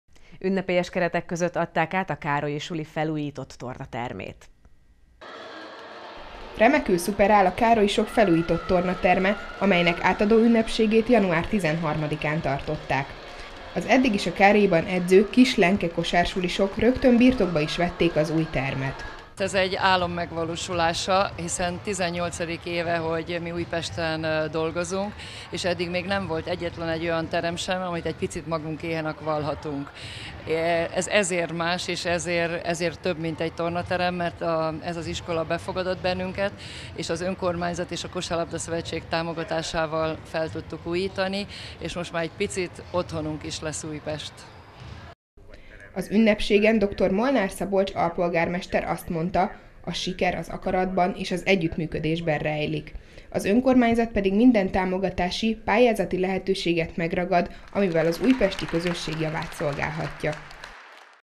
Átadták a Károlyi suli felújított tornatermét
Január 13-án hivatalosan is átadták a Károlyi suli felújított tornatermét. Az iskolában immáron a Kiss Lenke kosársuli kosarasai is remek körülmények között edzhetnek.